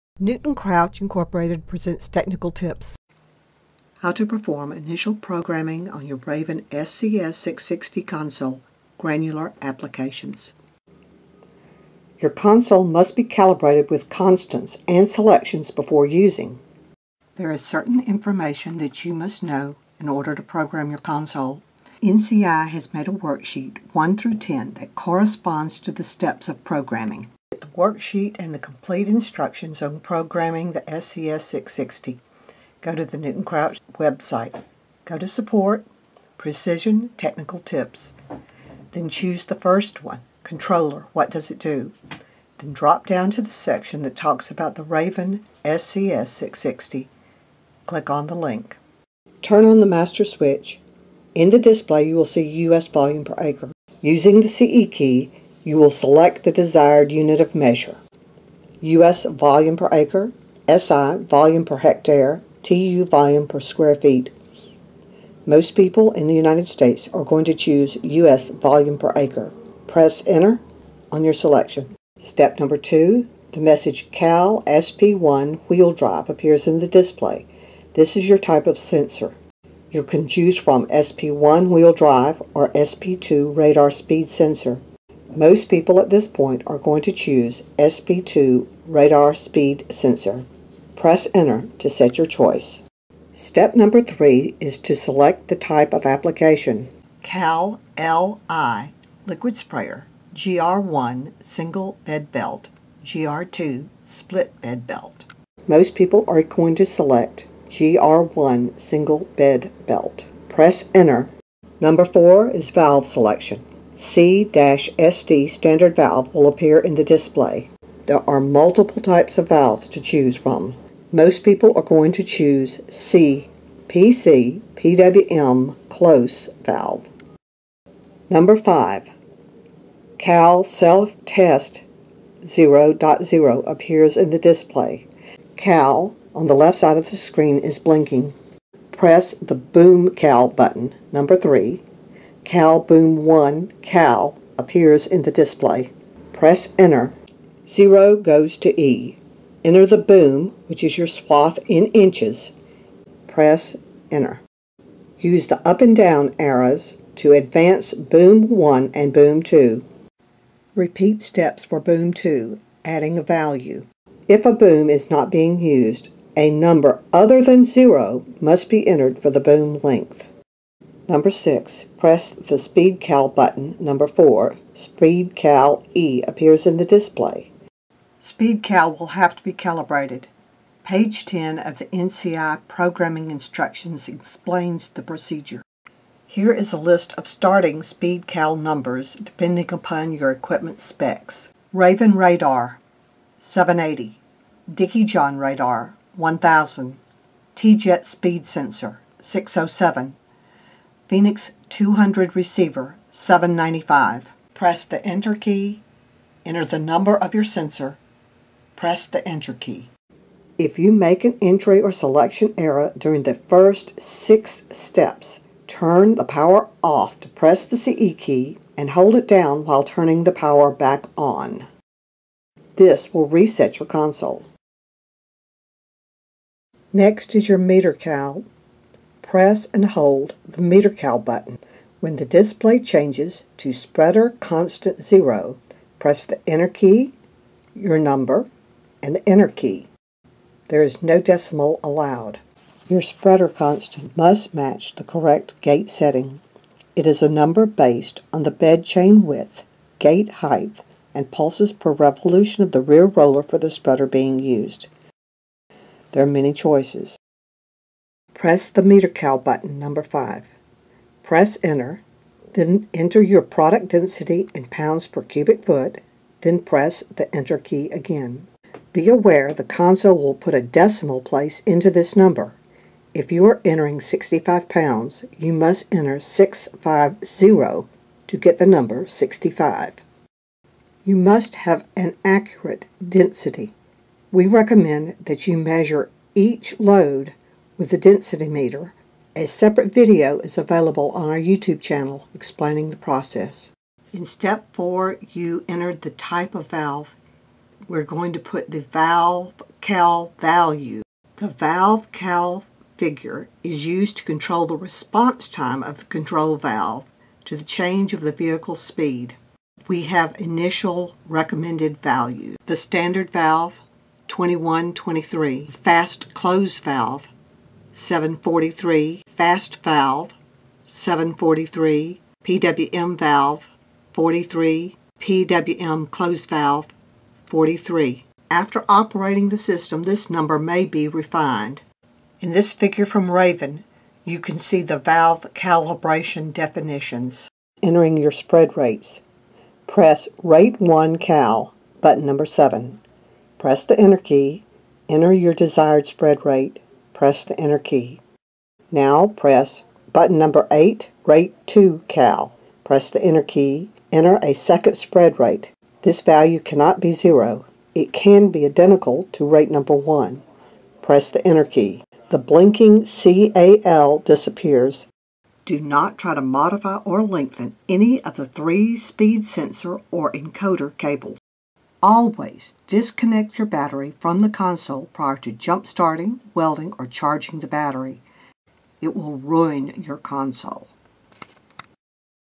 How to Program the Raven SCS 660
This video guides you step by step through the programming of a Raven SCS 660.  Download the worksheet from our website, follow the video instructions and setting up your console is easy.